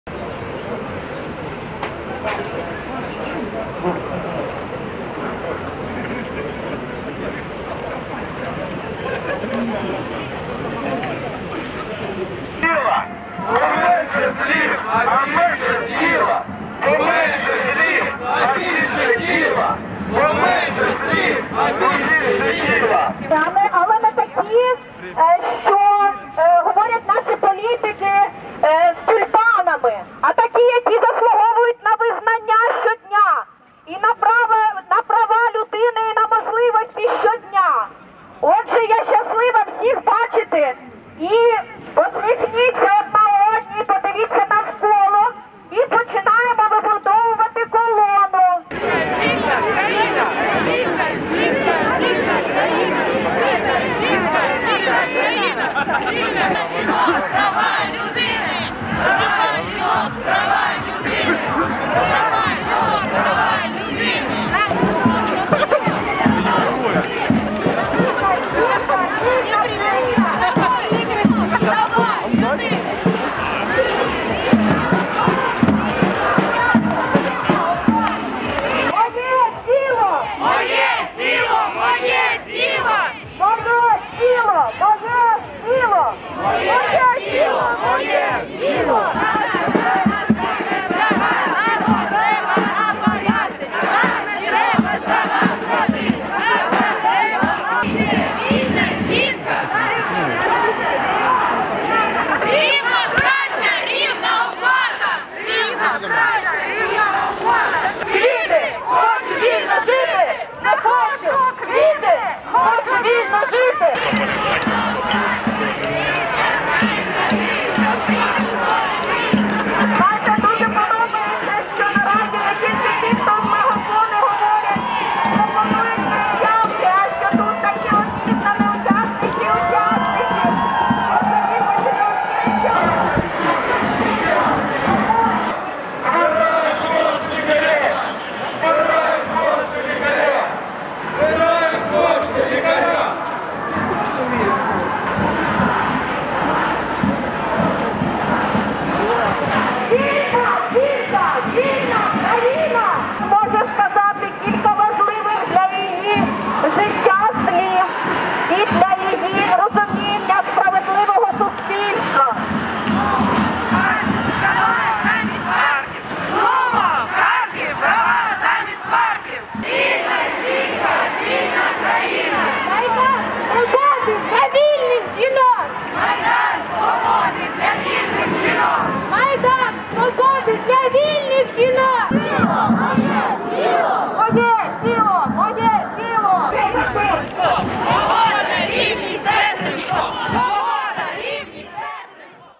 Марш жіночої солідарності
У Харкові на Марш ждіночої солідарності зібралося близькуо тисячі осіб.